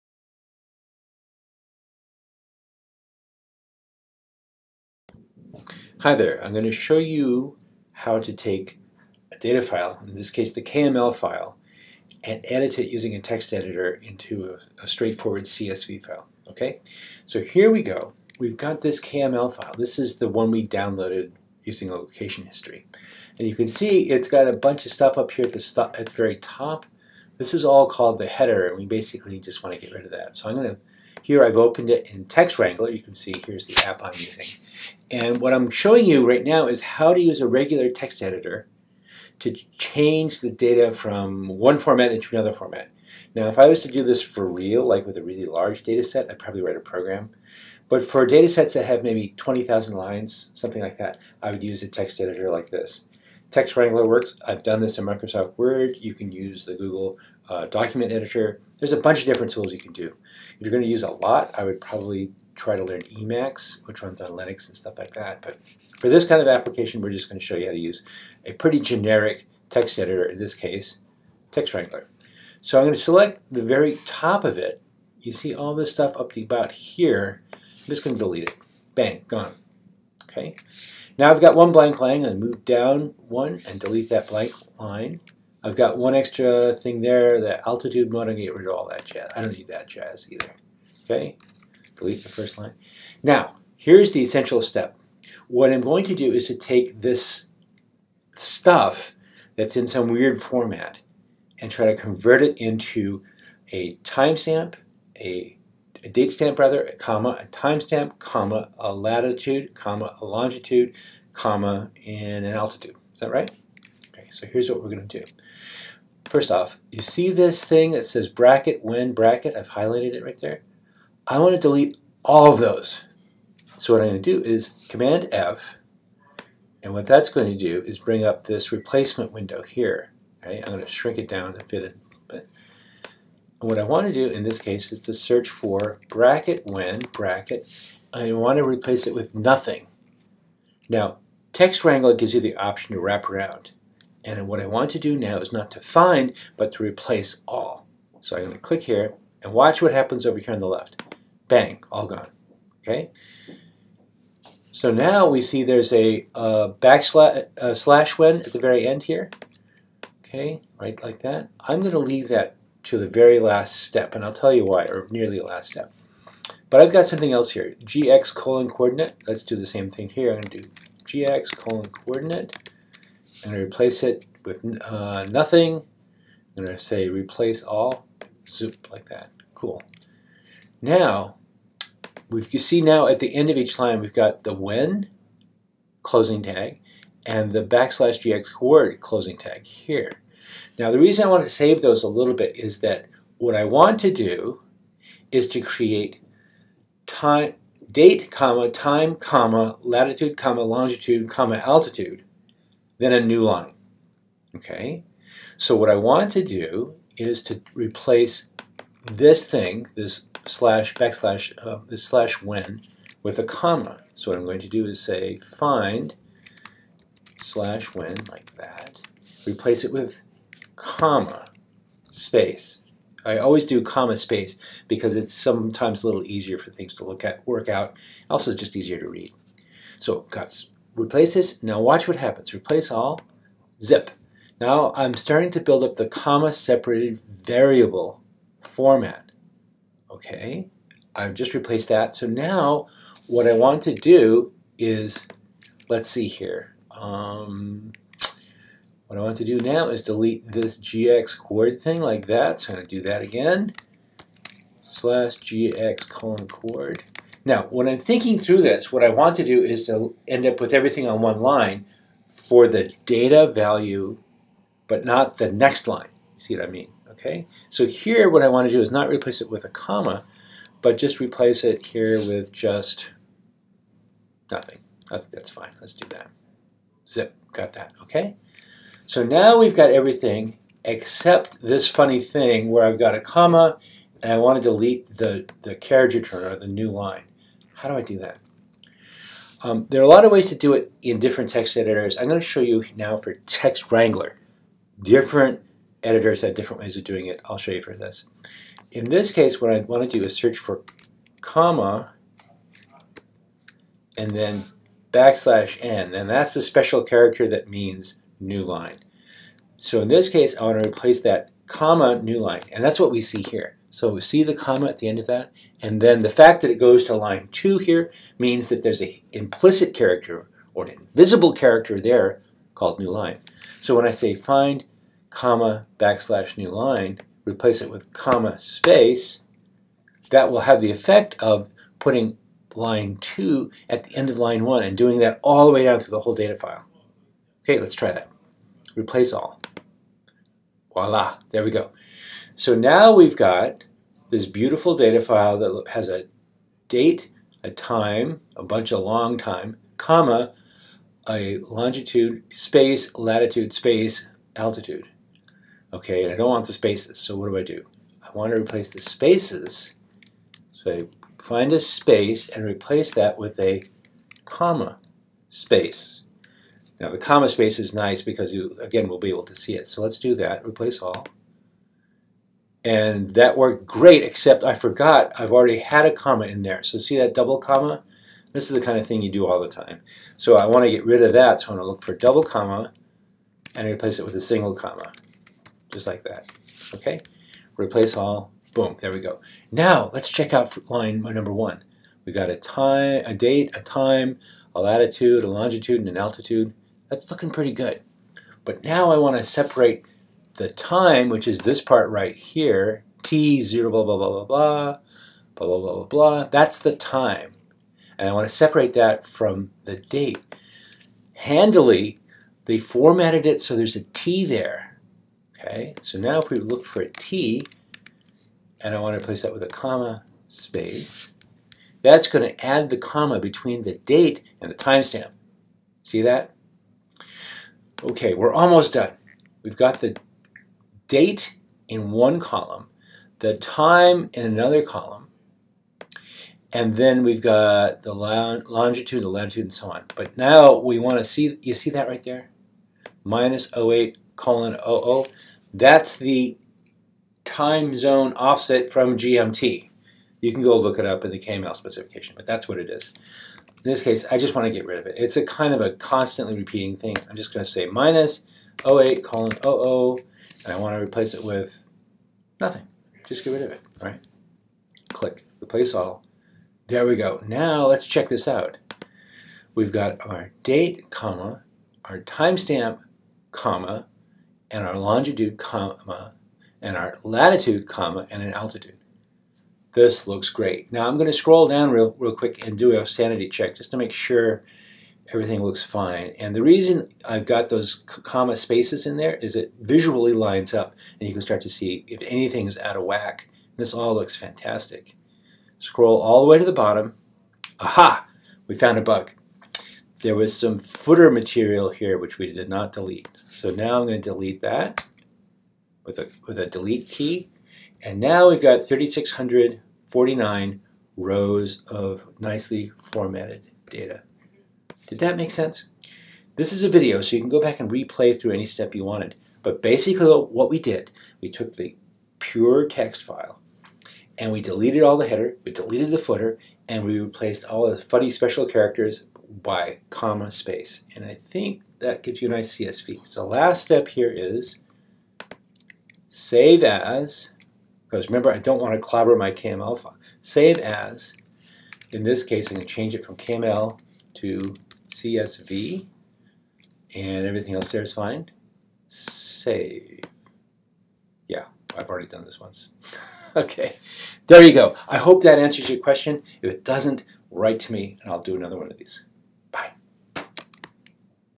Editing datafile into CSV
A demonstration of how to open a data file (in this case, a KML file) and convert it into a CSV file.